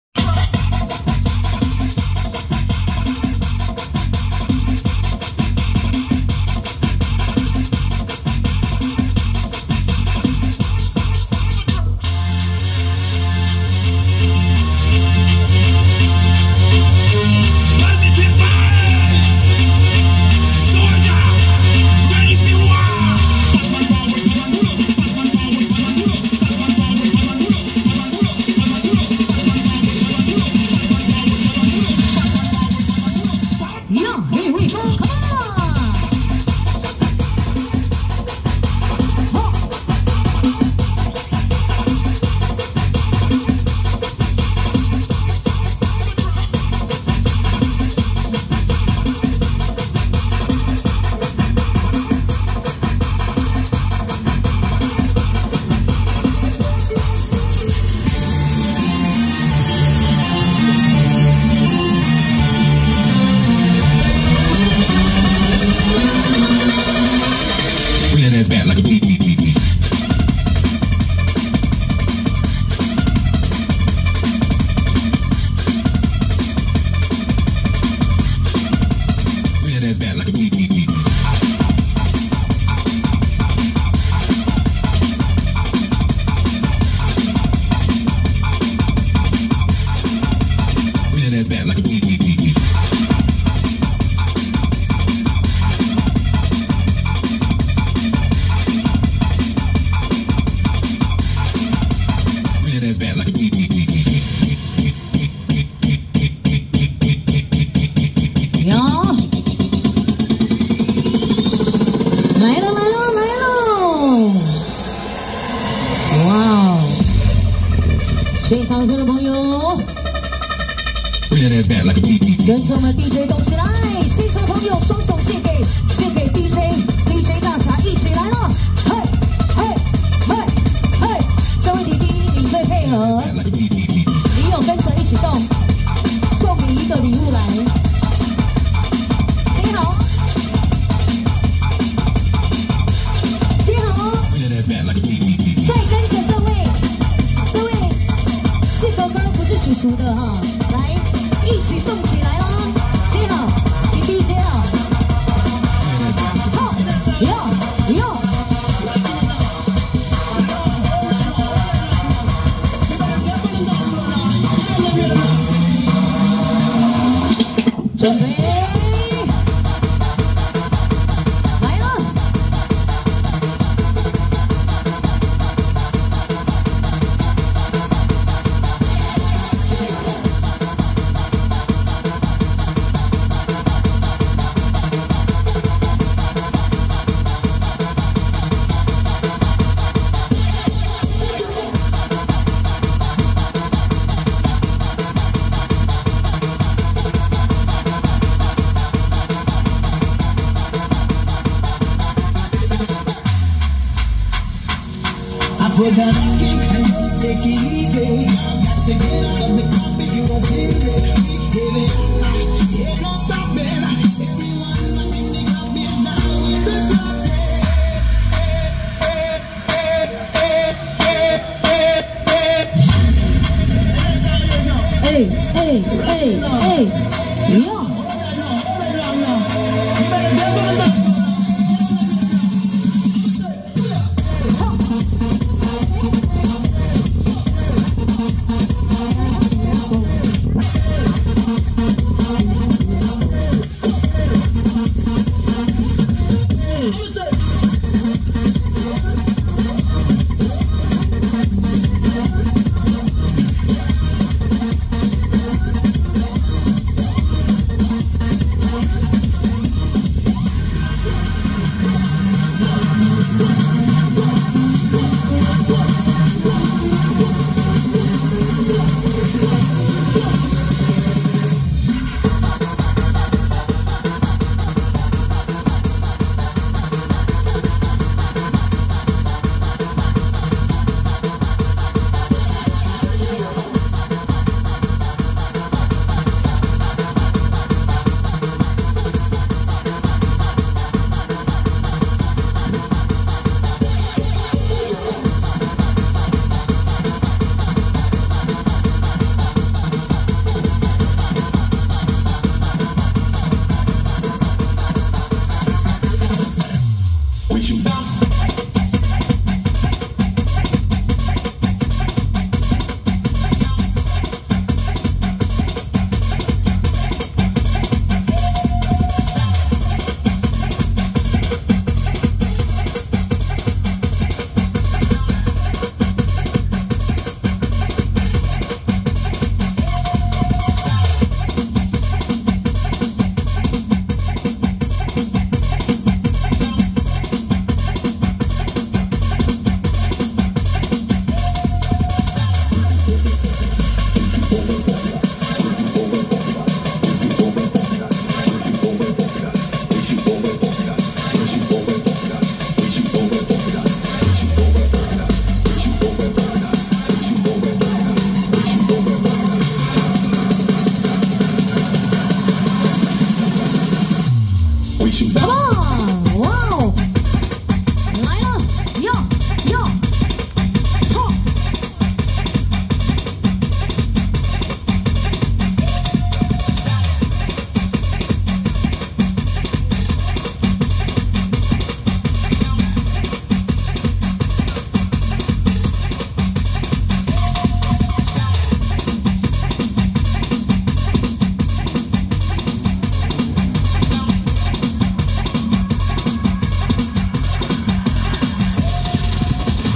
【DJ張大俠(新莊聖易堂紅壇舞台)】~113新莊地藏庵恭祝文武大眾老爺聖誕暗訪遶境(頭前區)
DJ張大俠(新莊聖易堂紅壇舞台)